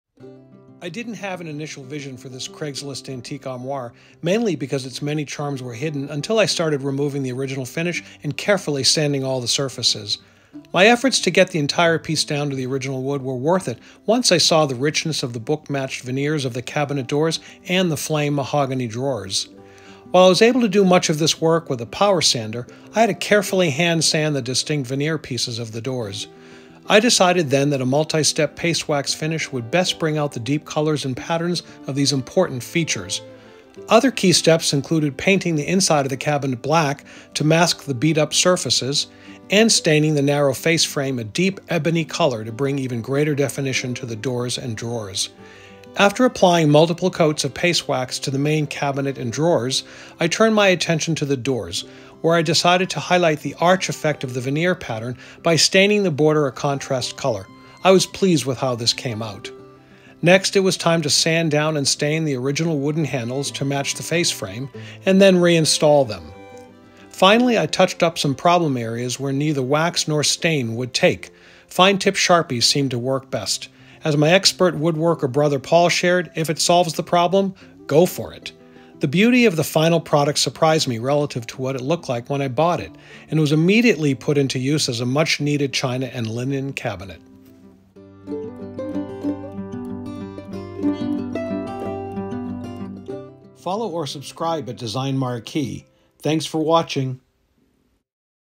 Armoire Adapted
Transforming a dingy antique armoire into a refined and practical piece that beautifully complements a mid-century dining room.